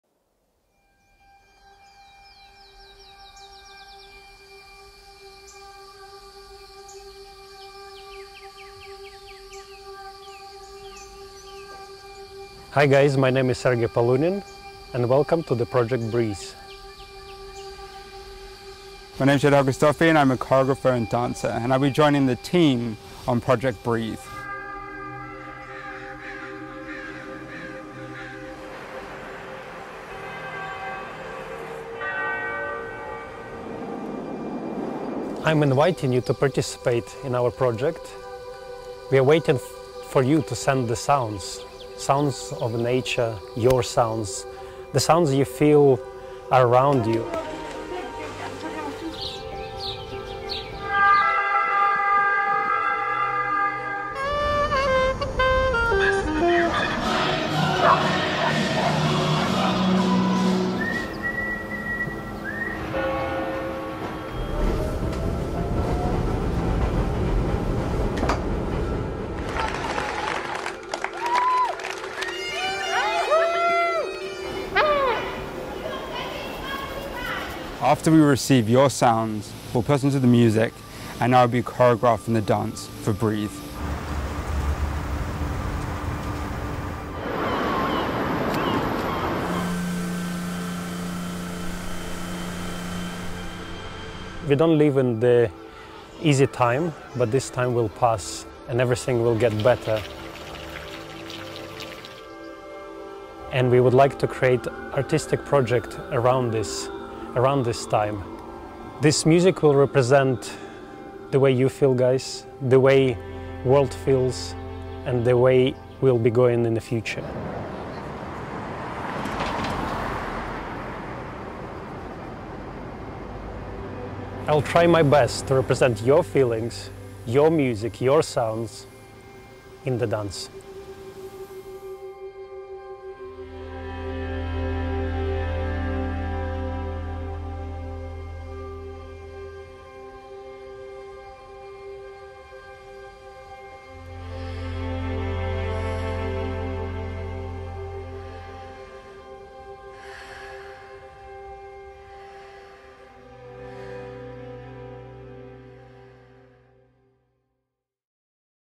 BREATHE Mir2theWorld [Русские субтитры]
Today’s world looks different from how we were used to see it. Many people are left alone with their fears, but now it is most important to unite. This is what BREATHE is for. It is not only about a collection of sounds. BREATHE unifies people during these difficult times, no matter where they are. 🌎

Сегодняшний мир выглядит не так, как мы привыкли. Многие остались один на один со своими страхами, но сейчас как никогда важно объединиться. Ради этого создан BREATHE. Это не просто звуки. BREATHE объединяет людей в трудные времена, где бы они ни находились. 🌍

Подробности о проекте вы можете найти, пройдя по ссылке 👇🏼👇🏼👇🏼